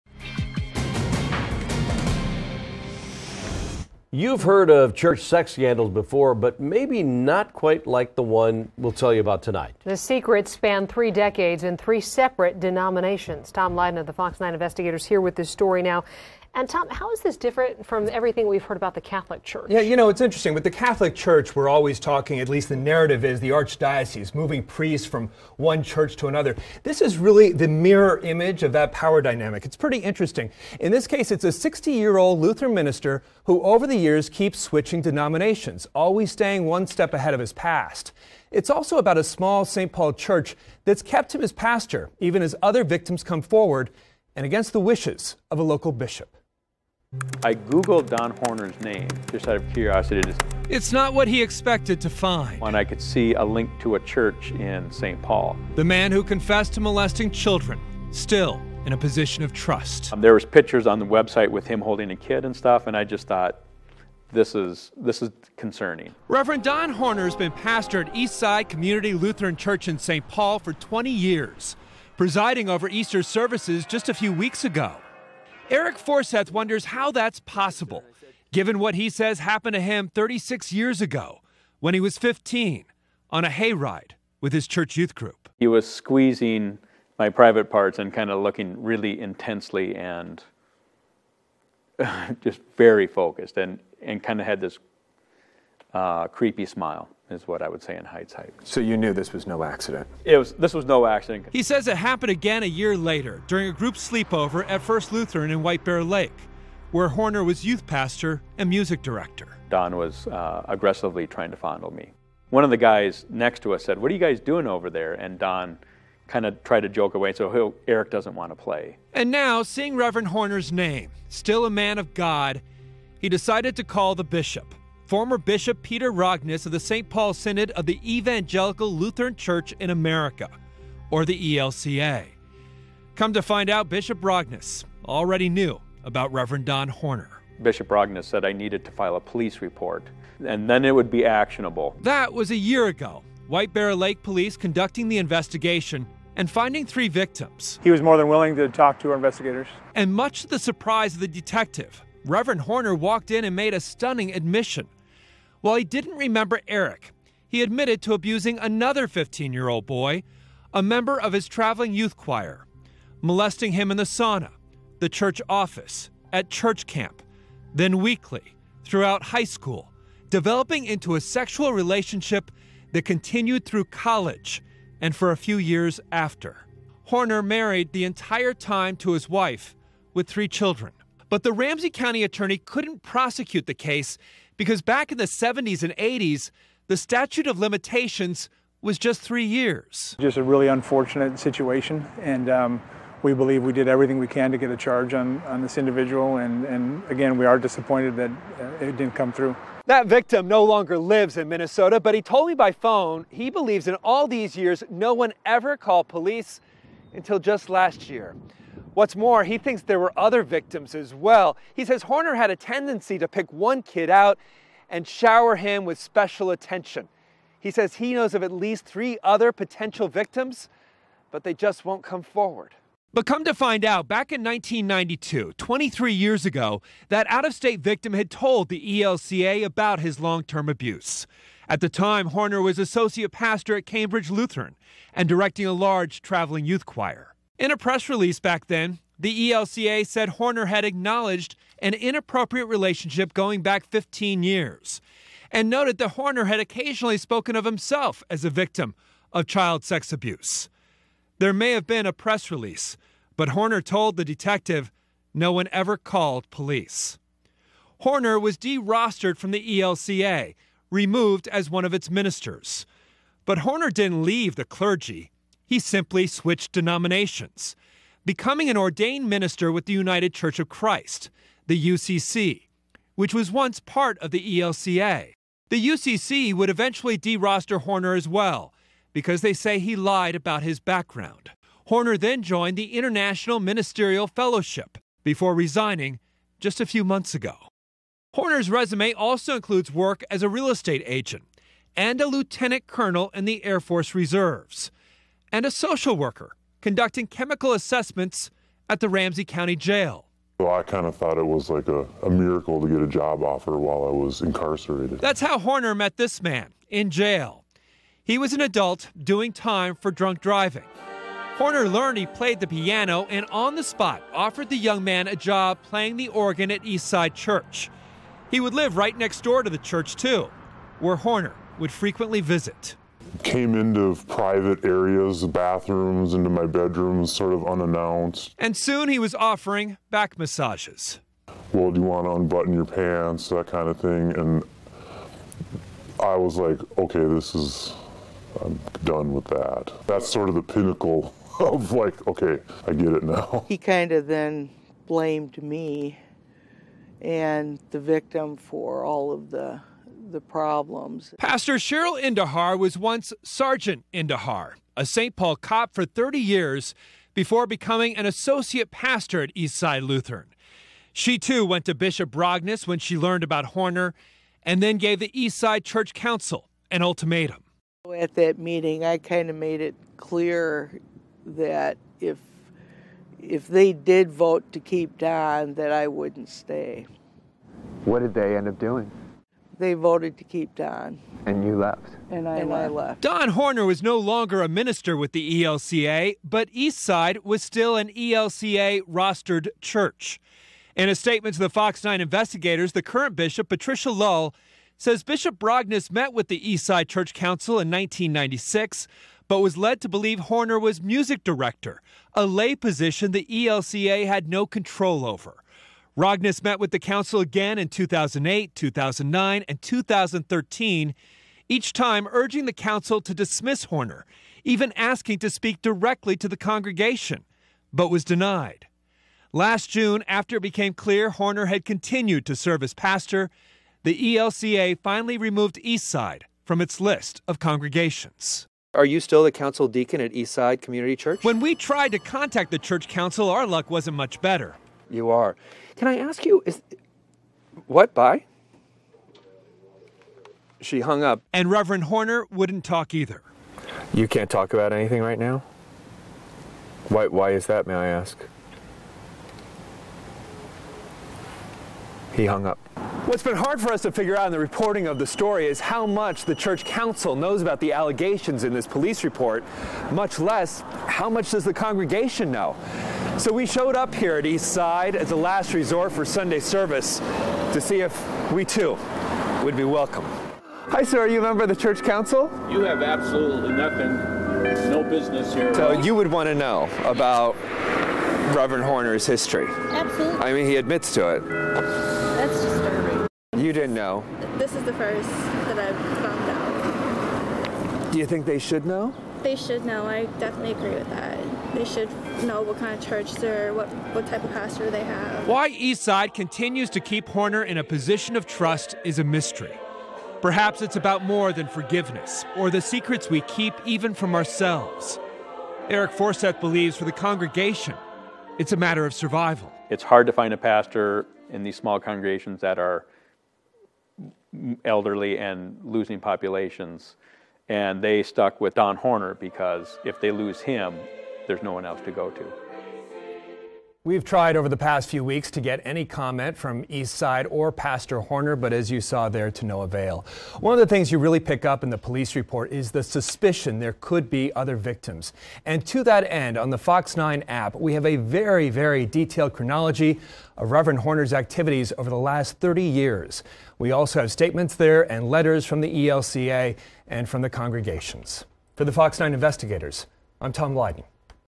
Blind Faith
A St. Paul church stands by their pastor, Rev. Don Horner, even though he was recently investigated three decades ago for abusing teenage boys, one of whom he admits to having an affair with. The story details how Horner changes religious denominations whenever confronted by his past. The story was broadcast on KMSP-TV, FOX 9, Minneapolis-St. Paul, MN on May 11, 2015.